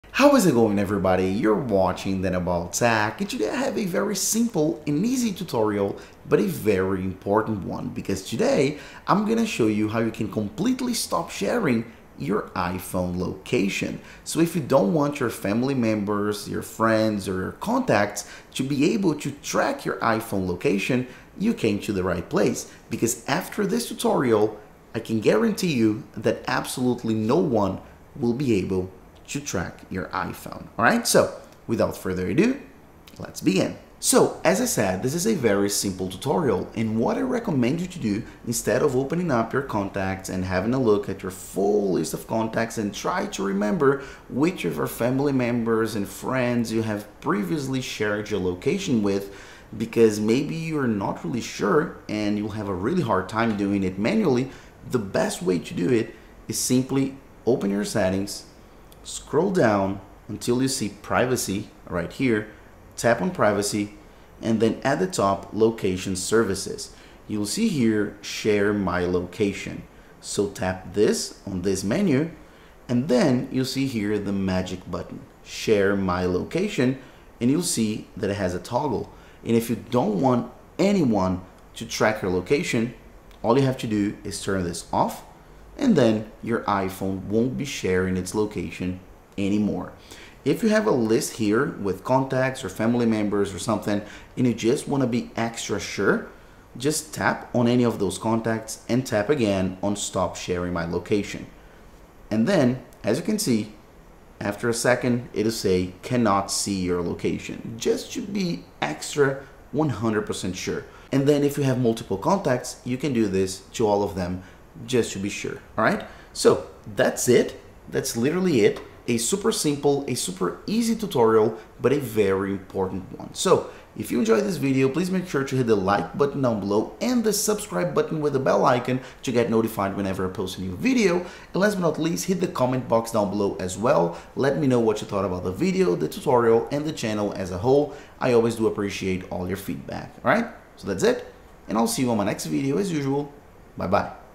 How to STOP Sharing your iPhone Location with your Contacts!
- Single-click stop sharing your location without Jailbreak[Quick & Safe]

In this video I teach you a very important Privacy Tutorial! How to stop sharing your iPhone Location with your Contacts. Follow my tutorial and no one will be able to track your iPhone! Hope U Enjoy!